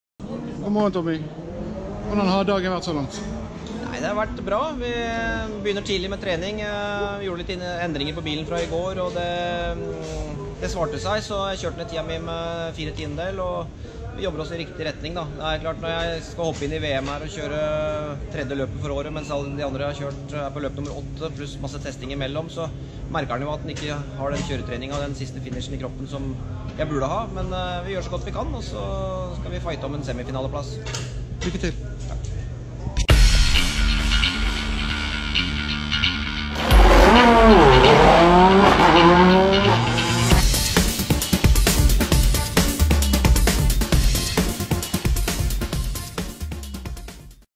3/9 Tommy Rustads siste løp: Tommy er positiv etter prøvene
Bretagne World RX of France - FIA World Rallycross Championship